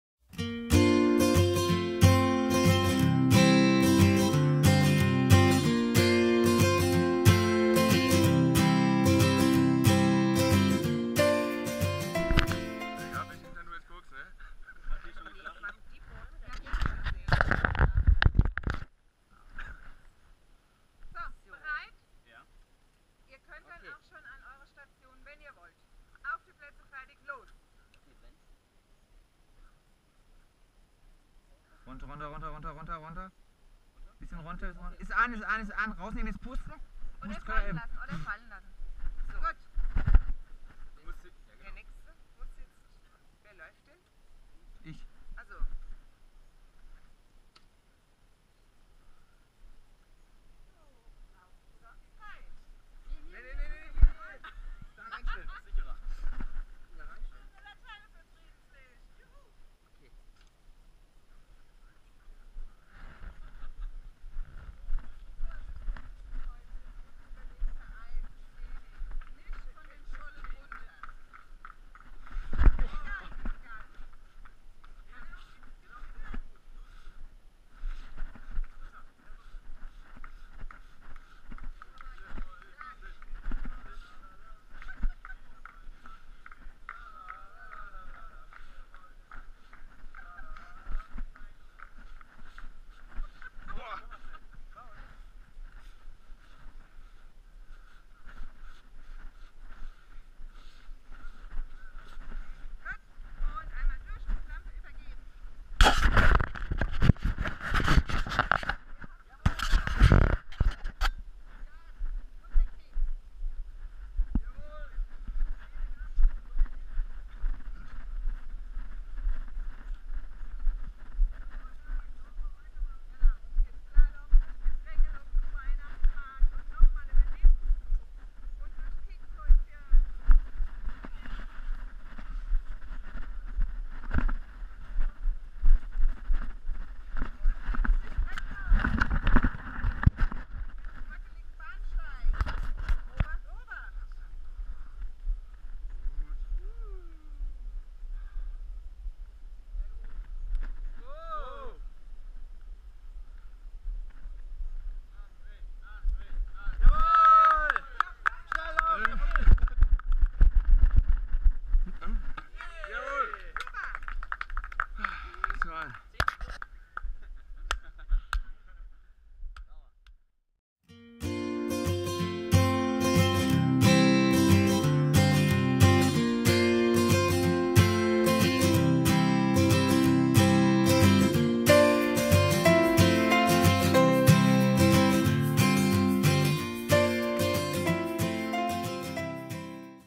Die Essener Stadtmusikanten an Station "Friedenslichter von Bad Orb"
Ein Video vom Spiel an der Station "Friedenslichter von Bad Orb" beim Ironscout 2014, aufgenommen mit unser GoPro befestigt am Stirnband!